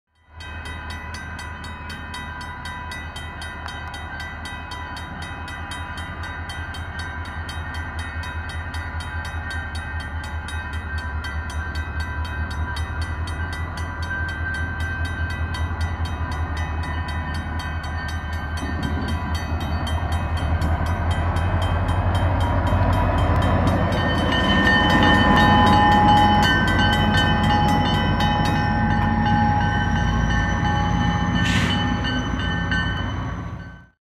CP #5764 running light at Marathon ON (11-May-2013)
2013 May 10, Marathon, Ontario
CP #5764 - SD40-2 (lead)